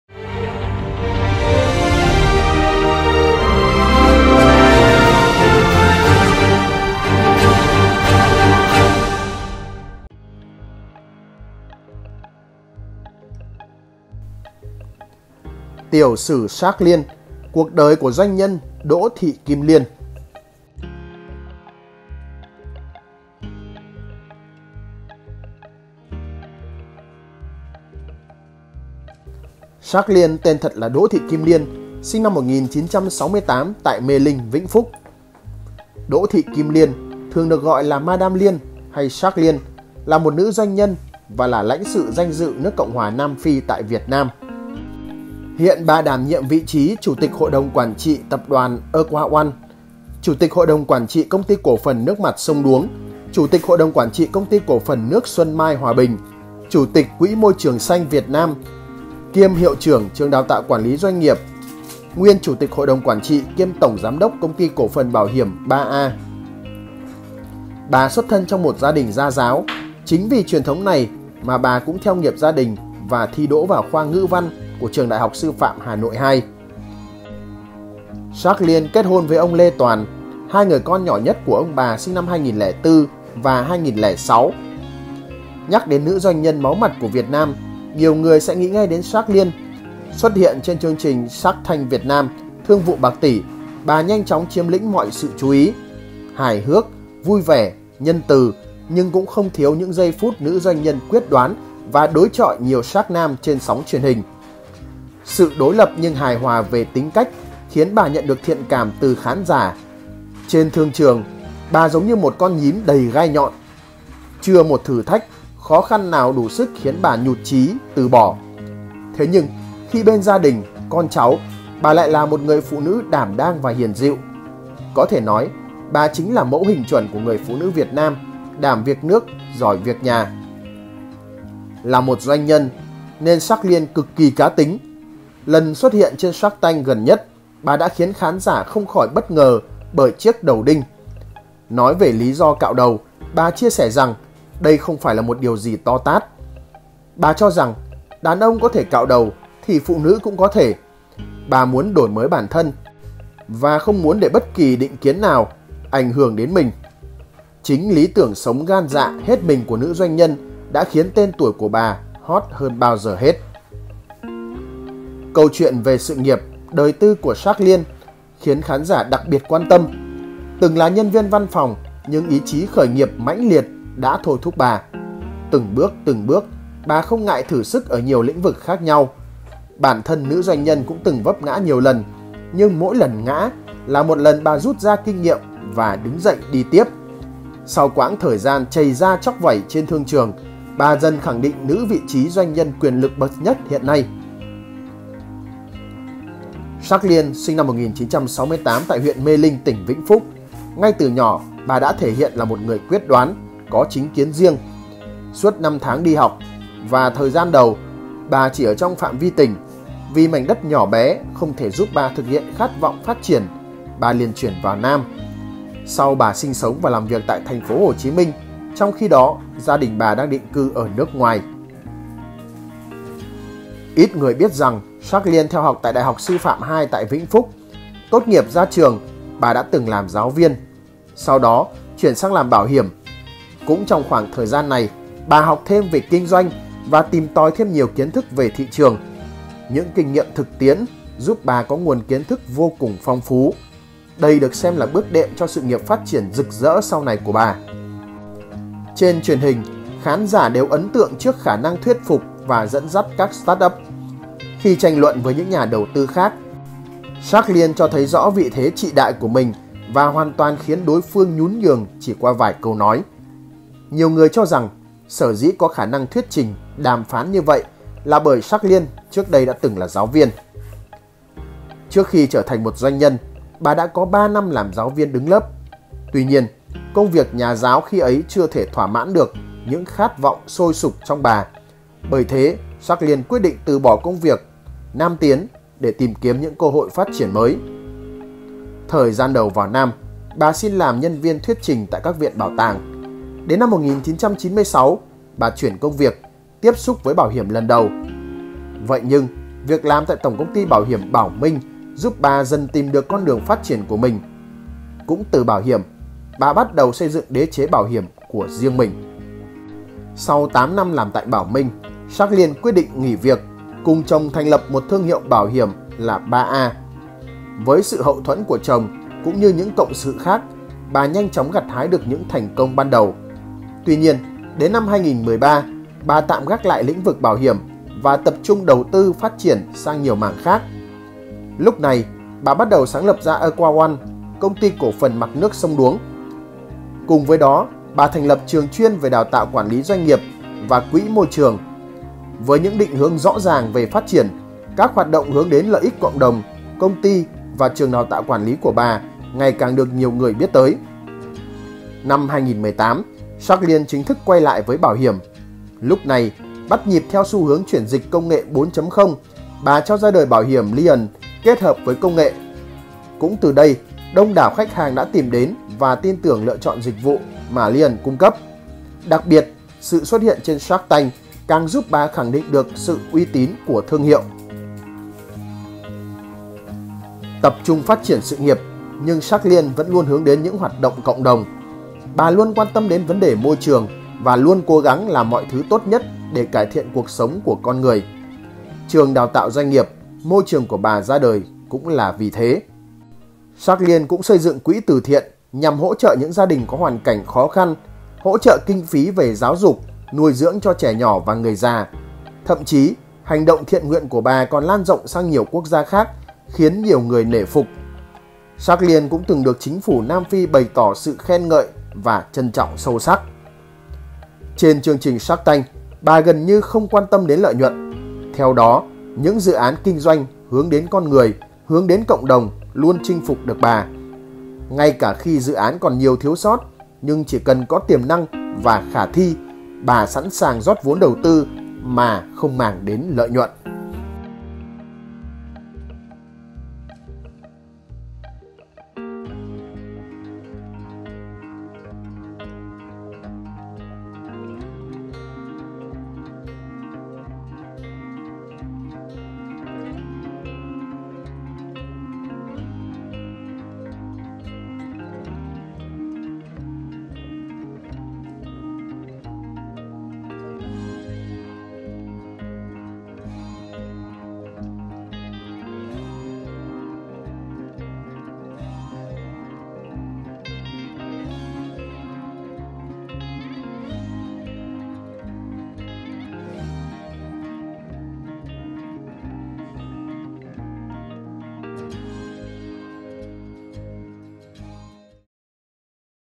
Tiểu sử SHARK LIÊN - Cuộc đời doanh nhân Đỗ Thị Kim Liên và cuộc hôn nhân với chồng đại gia
Shark Liên tên thật là Đỗ Thị Kim liên, sinh năm 1968, tại Mê Linh - Vĩnh Phúc.
Đỗ Thị Kim Liên (thường được gọi là Madam Liên, Shark Liên) là một nữ doanh nhân và là Lãnh sự Danh dự nước Cộng hoà Nam Phi tại Việt Nam. Hiện bà đảm nhiệm vị trí Chủ tịch Hội đồng Quản trị Tập đoàn AquaOne,Chủ tịch Hội đồng Quản trị Công ty Cổ phần nước mặt Sông Đuống, Chủ tịch Hội đồng Quản trị Công ty Cổ phần Nước Xuân Mai - Hoà Bình,Chủ tịch Quỹ Môi trường Xanh Việt Nam (Green Vietnam Fund), kiêm Hiệu trưởng trường Đào tạo Quản lý Doanh nghiệp (CBAM),[3] nguyên Chủ tịch Hội đồng Quản trị kiêm Tổng giám đốc Công ty Cổ phần Bảo hiểm AAA.
Bà xuất thân trong một gia đình nhà giáo, chính vì truyền thống này mà bà cũng theo nghiệp gia đình thi và đỗ vào khoa Ngữ Văn của Đại học Sư Phạm Hà Nội II. Bà kết hôn với ông Lê Toàn. Hai người con nhỏ nhất của ông bà sinh năm 2004 và 2006.
Nhắc đến nữ doanh nhân máu mặt của Việt Nam, nhiều người sẽ nghĩ ngay đến Shark Liên. Xuất hiện trên chương trình Shark Tank Việt Nam – Thương vụ bạc tỷ, bà nhanh chóng chiếm lĩnh mọi sự chú ý. Hài hước, vui vẻ, nhân từ nhưng cũng không thiếu những giây phút nữ doanh nhân quyết đoán và đối chọi nhiều Shark nam trên sóng truyền hình.
Sự đối lập nhưng hài hòa về tính cách khiến bà nhận được thiện cảm từ khán giả. Trên thương trường, bà giống như một con nhím đầy gai nhọn. Chưa một thử thách, khó khăn nào đủ sức khiến bà nhụt trí, từ bỏ. Thế nhưng khi bên gia đình, con cháu, bà lại là một người phụ nữ đảm đang và hiền dịu. Có thể nói, bà chính là mẫu hình chuẩn của người phụ nữ Việt Nam: Đảm việc nước – Giỏi việc nhà.
Là doanh nhân nhưng Shark Liên lại cực kỳ cá tính. Lần xuất hiện trên Shark Tank gần nhất, bà đã khiến khán giả không khỏi bất ngờ bởi chiếc đầu đinh. Nói về lí do cạo đầu, bà chia sẻ rằng đây không phải là một điều to tát. Bà cho rằng, đàn ông có thể cạo đầu thì phụ nữ cũng có thể. Bà muốn đổi mới bản thân và không muốn để bất cứ định kiến nào có thể ảnh hưởng đến mình. Chính lý tưởng sống ga dạ, hết mình của nữ doanh nhân càng khiến tên tuổi của bà hot hơn bao giờ hết.